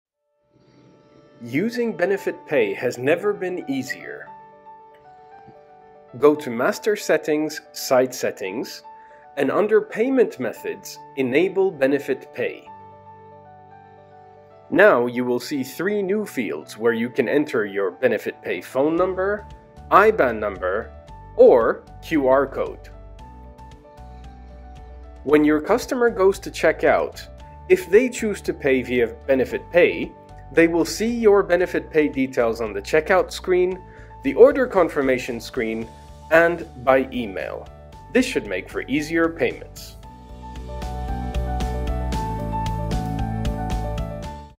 Alligator - BenefitPay
Alligator is an eCommerce solution that will meet your every need! It’s quick, easy to use and you can literally start selling your products within 15 minutes!

We take zero commission on your sales… forever! It includes all the features and flexibility you can imagine, and dedicated year-round support, all for and extremely affordable annual subscription!